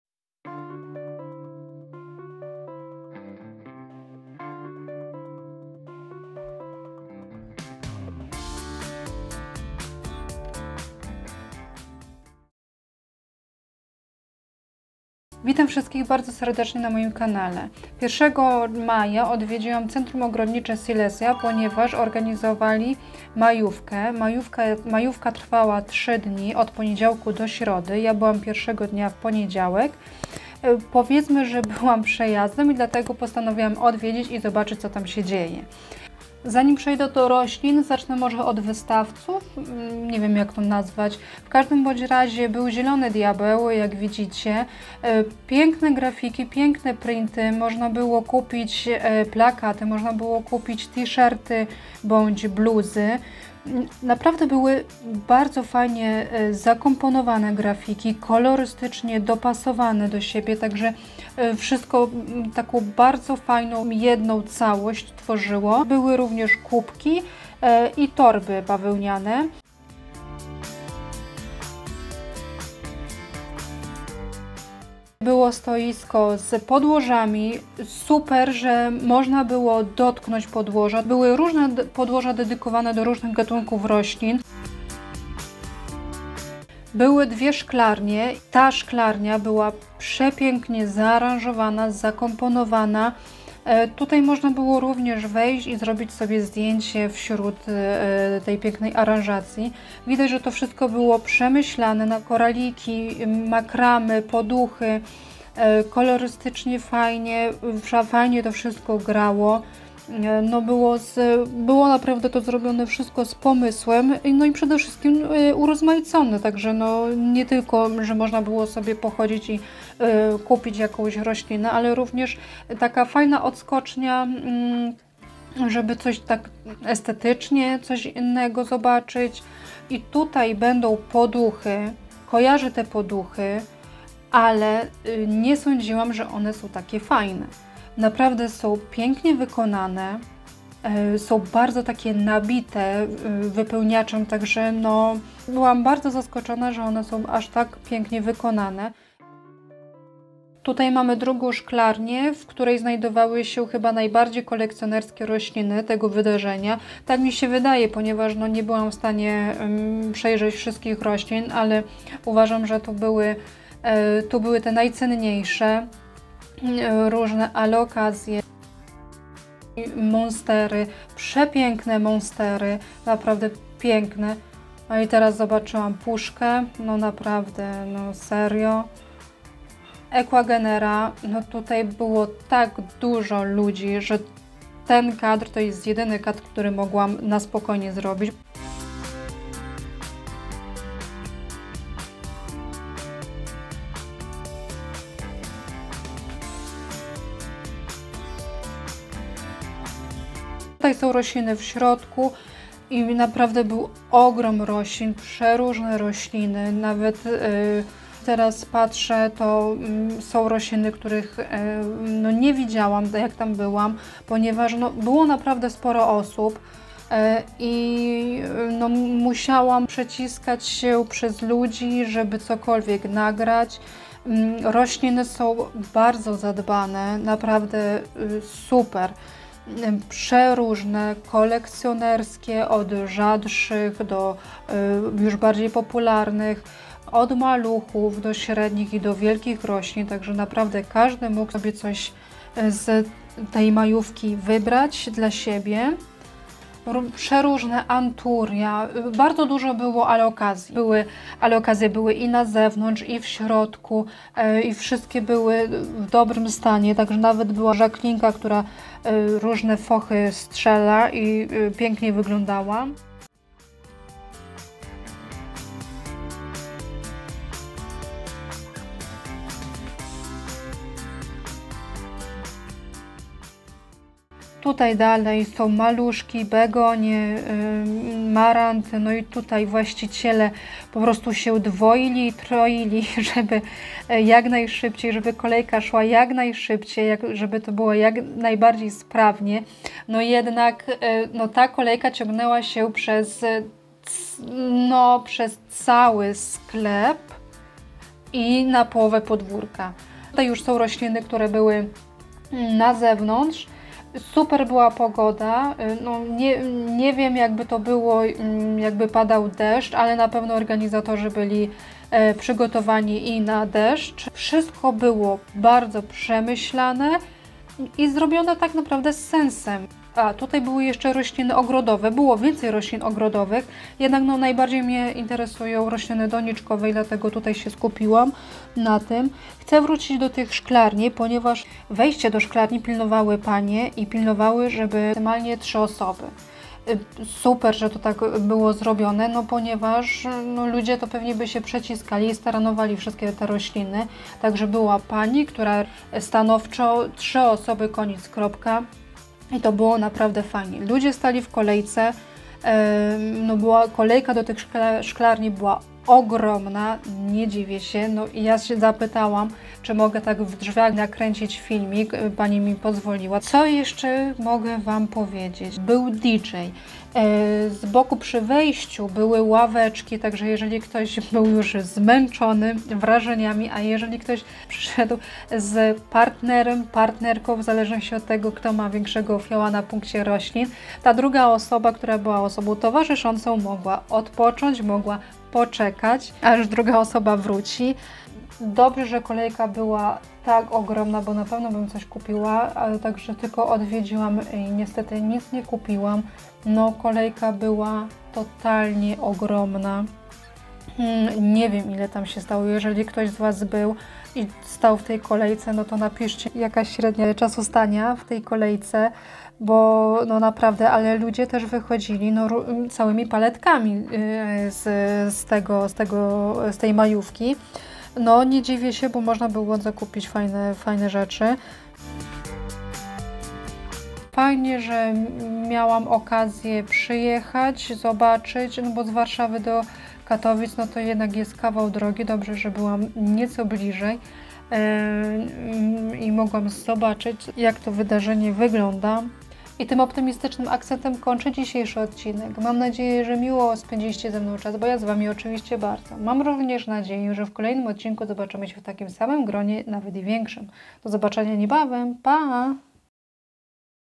"Odlotowa Majówka z CO Silesia" | Byłam, widziałam i nagrałam 😉
Hej, Hej 💚
Dziękuję, że odwiedziłeś/aś mój kanał. Zapraszam na relację z majóweczki, którą organizowało Centrum Ogrodnicze Silesia 🌞

Miłego oglądania!!! :)